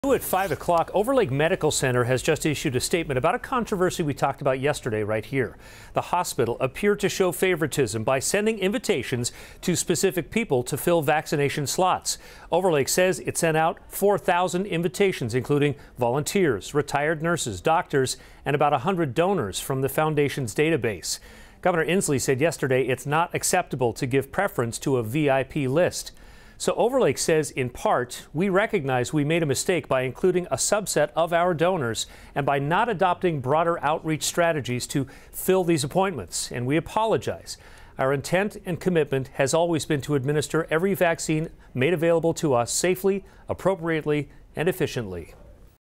Bellevue's Overlake Medical Center apologizes for prioritizing eligible donors for COVID-19 vaccine
Overlake Medical Center in Bellevue is apologizing for how it handled its COVID-19 vaccine distribution plan, by prioritizing eligible donors and "by not adopting a broader outreach strategy."

On Jan. 22, the hospital said it emailed nearly everyone on file to fill unclaimed vaccination slots on short notice.

Overlake said "proactive communications were sent to those for whom we had email addresses, including all of our 3,300 employees, patients aged 65 and above, groups of retired nurses and physicians and our volunteer and donor communities.  All of those individuals were sent a communication with a separate scheduling link due to various limitations of our schedule system.”

During his news conference Tuesday afternoon, Gov. Jay Inlsee expressed his disdain for Overlake's actions.